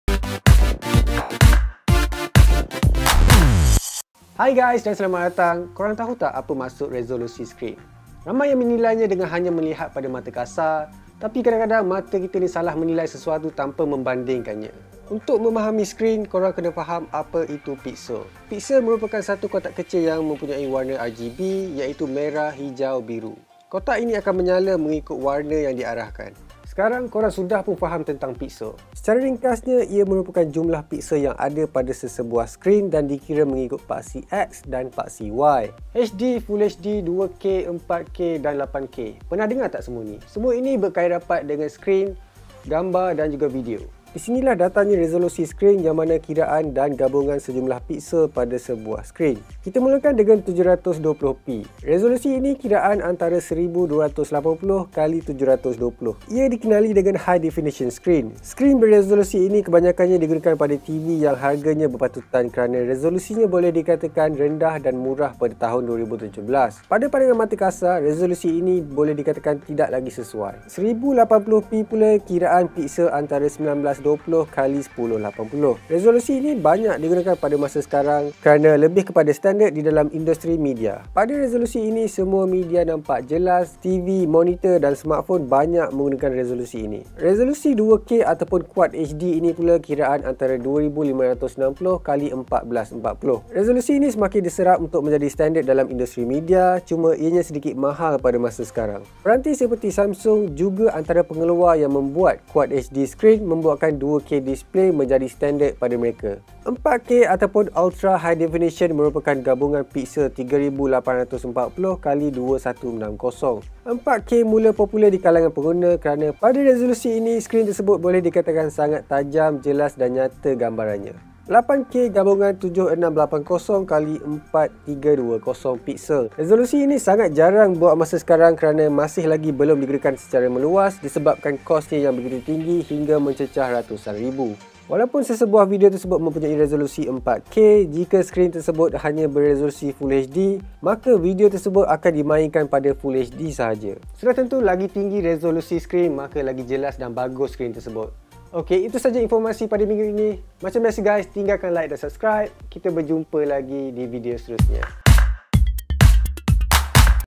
Apakah Itu Piksel Dan Resolusi Skrin?
Piksel dan resolusi skrin adalah salah satu perkara yang sering diperkatakan apabila berbicara mengenai pelbagai gajet dan peranti pada masa kini. Tetapi apakah disebaliknya? Jom tonton penerangan ringkas mengenai kedua-dua istilah ini.